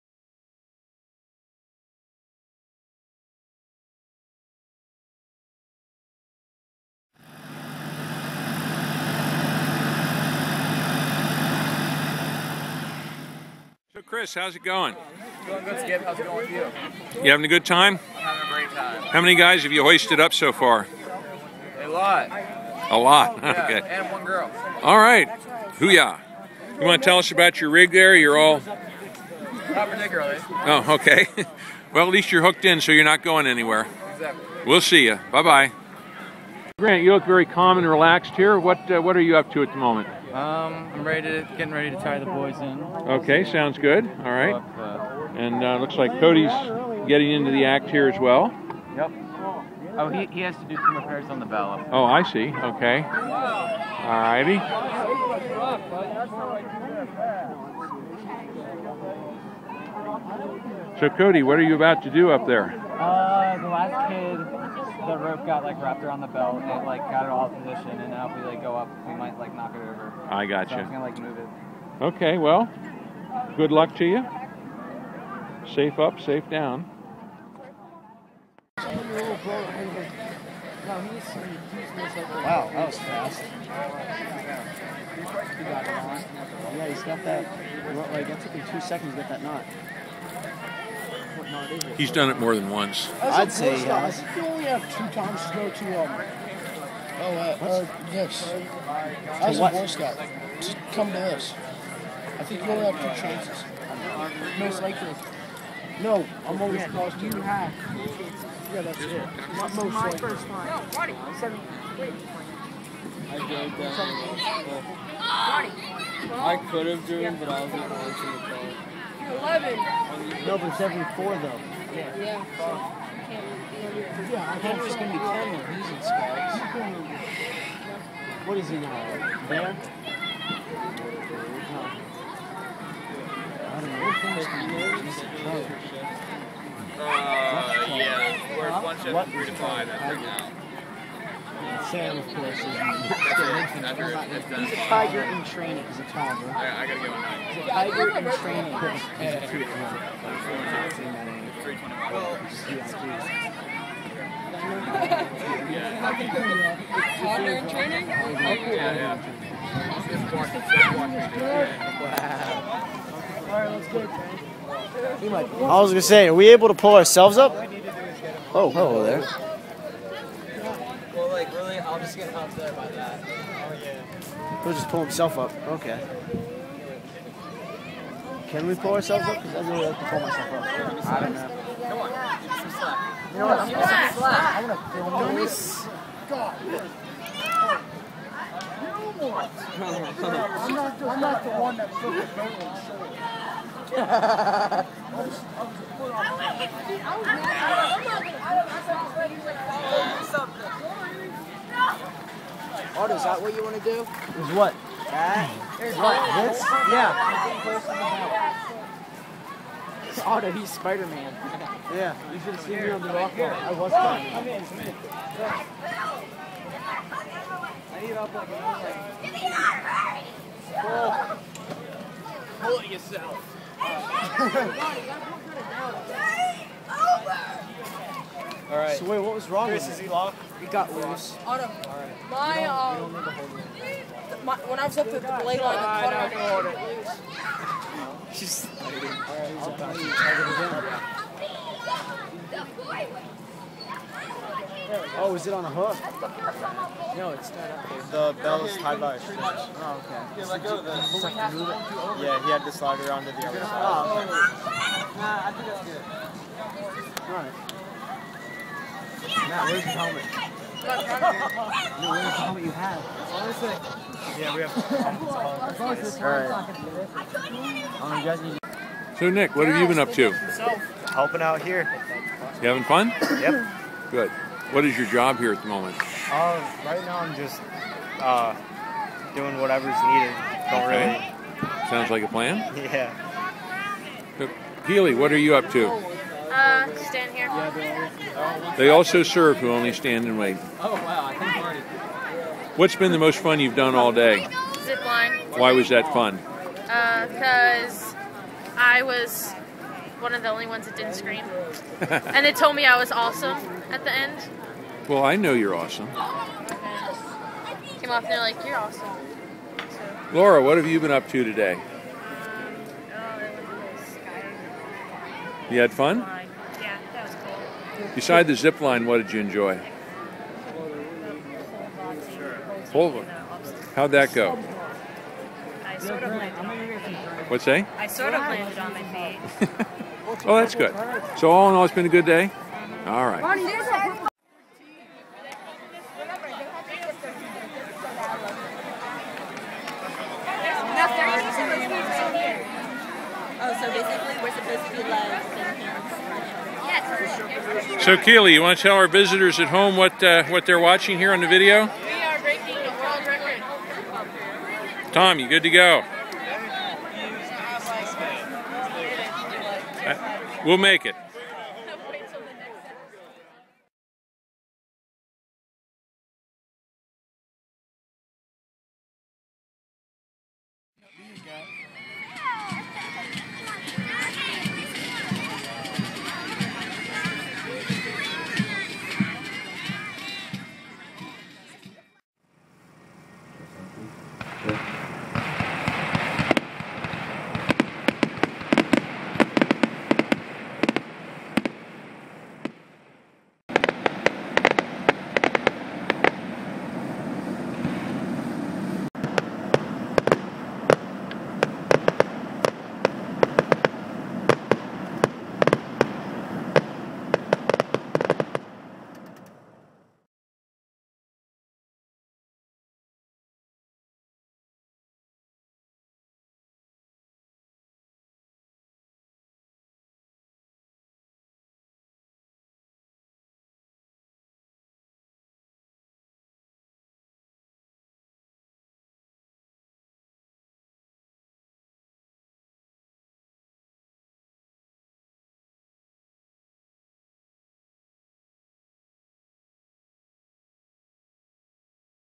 Sea Scout Ship 25 helps staff the Bicentennial Wizard Safari - Part 2
Part 2 - Sea Scout Ship 25 helped staff the Bicentennial Wizard Safari of the New Birth of Freedom Council, BSA, at Wizard Ranch, Hellam Township, York County, PA October 1-3, 2010.